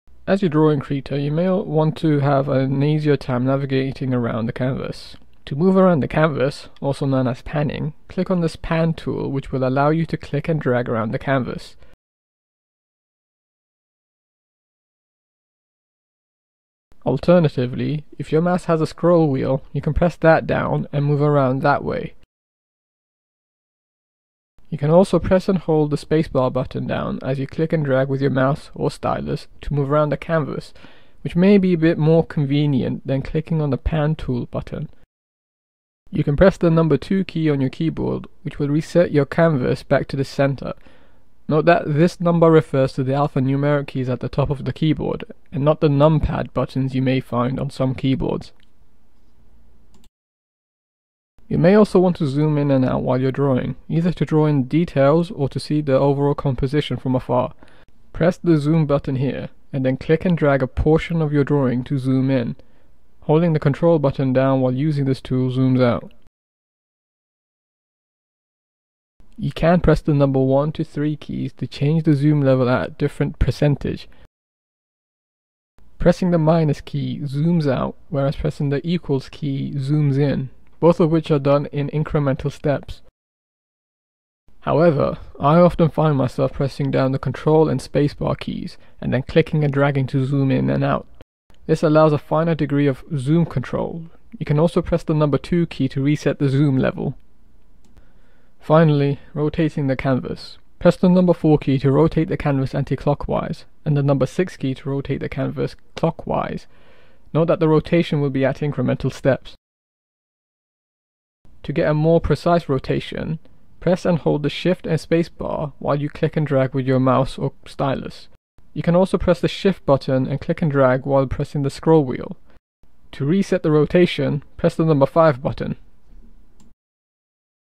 Krita - Canvas controls
Video showing the keyboard keys to moving around the canvas in Krita

CORRECTION: At around the 0:42 mark, I mention a shortcut to reset the canvas view being accessible via the alphanumeric key and not the number pad. You can use the number pad to reset the view!

Contents:
0:06 - Panning around the canvas
0:51 - Zooming in and out
1:39 - Rotating the canvas